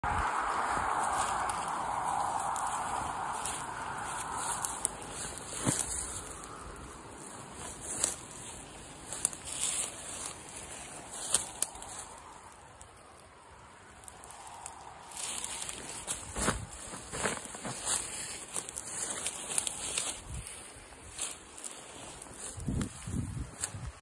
Never seen so many puffball mushrooms have you?
White ones are good to eat the brown ones are releasing spores ready for next years mushrooms